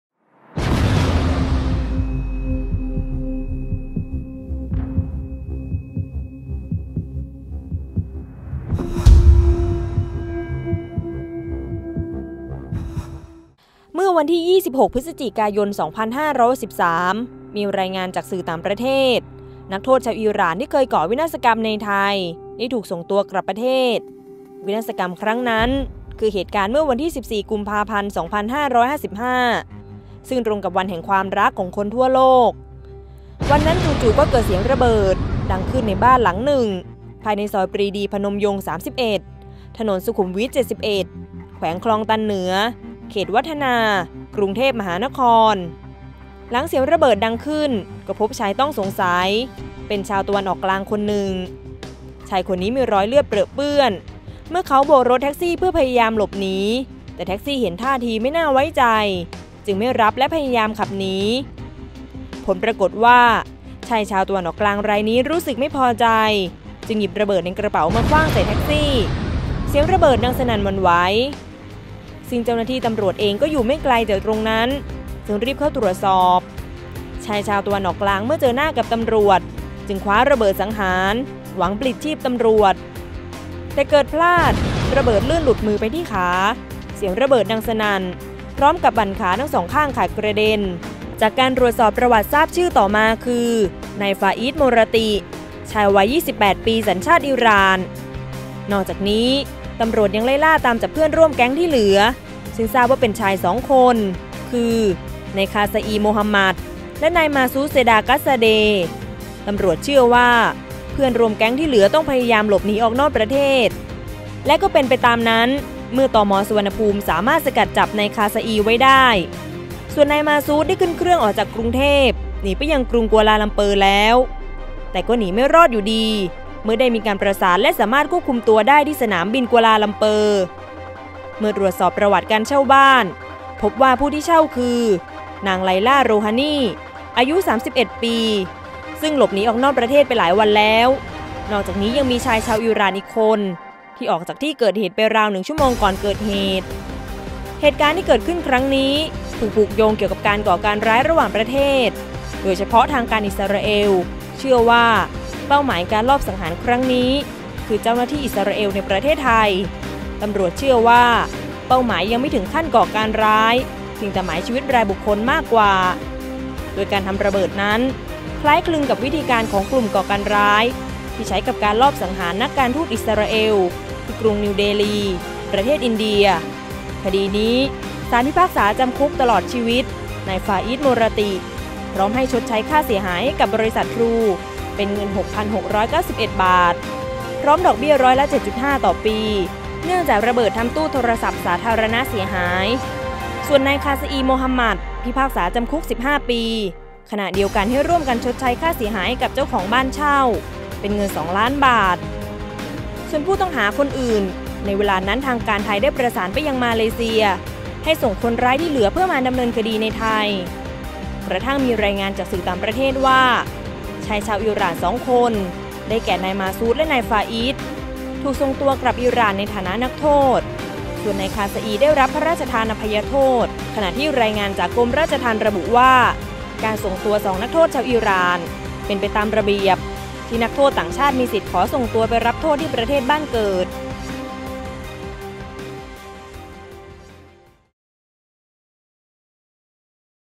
3 นาทีคดีดัง : แผนแตก บึมขาขาด แก๊งอิหร่านเขย่าขวัญคนกรุง | Thairath Online
เมื่อวันที่ 26 พ.ย.2563 มีรายงานจากสื่อต่างประเทศ นักโทษชาวอิหร่าน ที่เคยก่อวินาศกรรมในไทย ได้ถูกส่งตัวกลับประเทศ วินาศกรรมครั้งนั้น คือ เหตุการณ์ เมื่อวันที่ 14 กุมภาพันธ์ 2555 ซึ่งตรงกับวันแห่งความรักของคนทั่วโลก

3 นาทีคดีดัง โดย ทีมข่าวเฉพาะกิจไทยรัฐออนไลน์ จะพาทุกท่านไปย้อนรอยเหตุการณ์ที่เกิดขึ้น เมื่อ 8 ปีก่อน ซึ่งจุดเริ่มต้นนั้นถือเป็นความโชคดี ที่กลุ่มผู้ร้ายได้ทำพลาด เป็นที่มาของแผนการร้ายที่เตรียมไว้ถูกเปิดเผยขึ้น